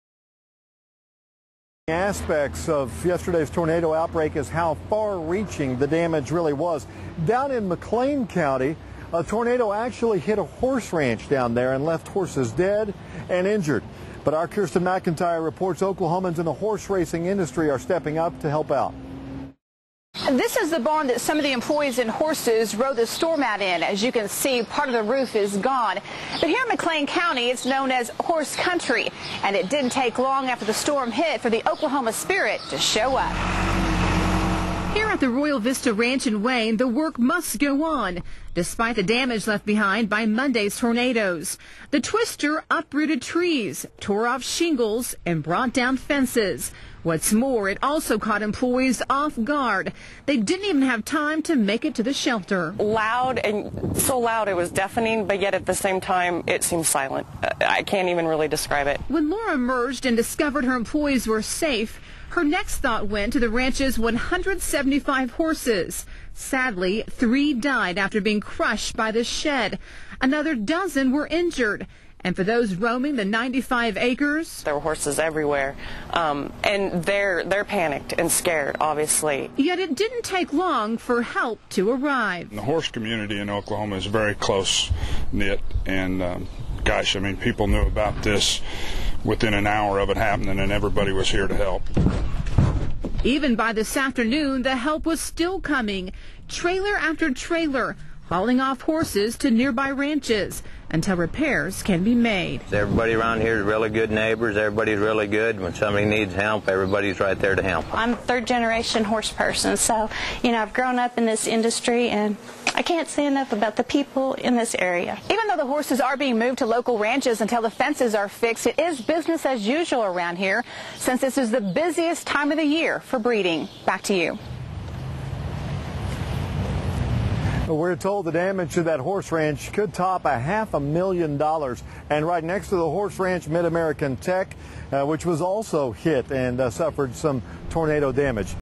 Horse Community Helps Ranch in Time of Need After Storm
A twister hit a horse ranch in McClain County Monday, leaving three horses dead and about a dozen others injured. However, those who work at the ranch said they are counting their blessings.